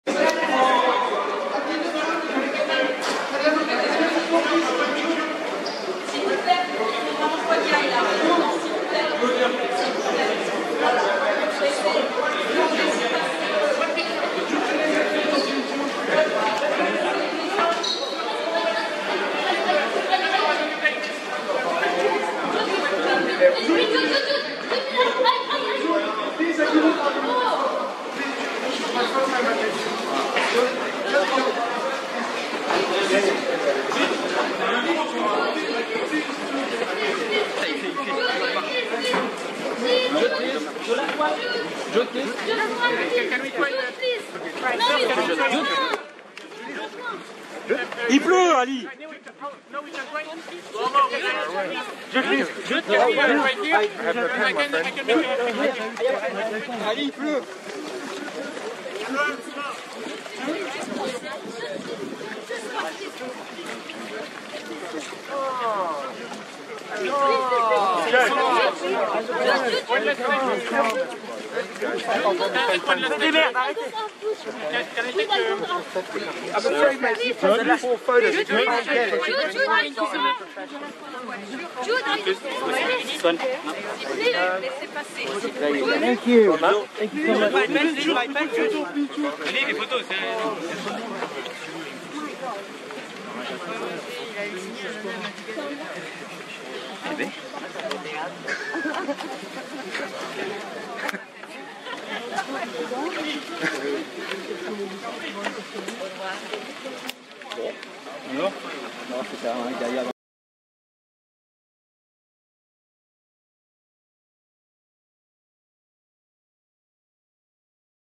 Jude Law AVANT PREMIERE (LE JEU DE LA REINE) PARIS 11 MARS 2024
Jude Law est un Acteur, Producteur, Producteur délégué britannique.UGC CINÉ CITÉ LES HALLES